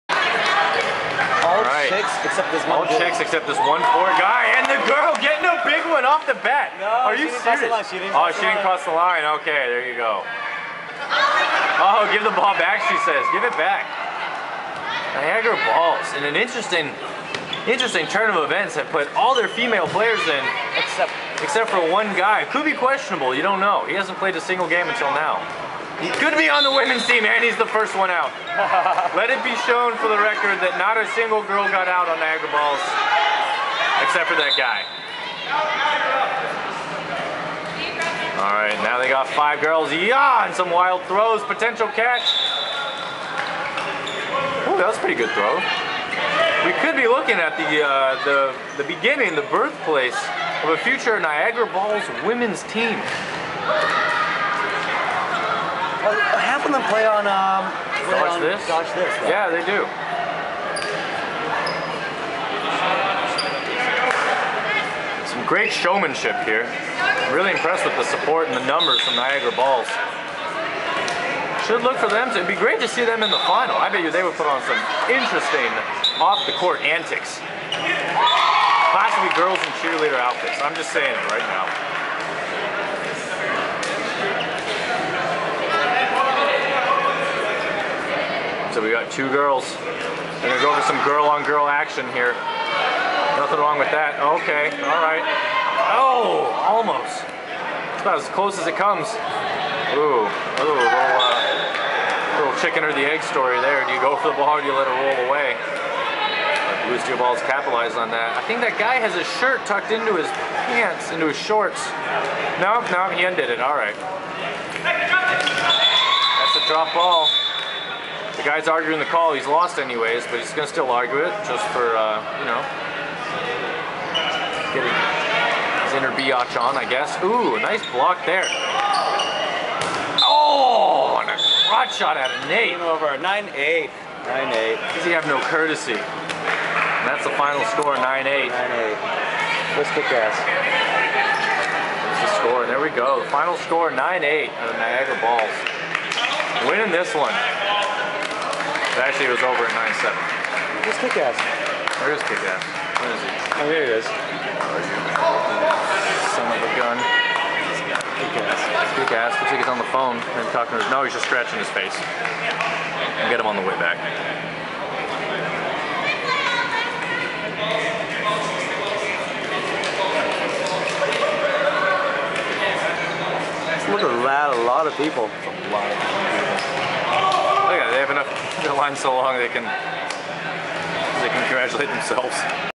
Niagara Balls vs Blue Steel 17 of 17
Blue Steel Balls win last set.

Finals Score: Niagara Balls 9 - Blue Steel Balls 8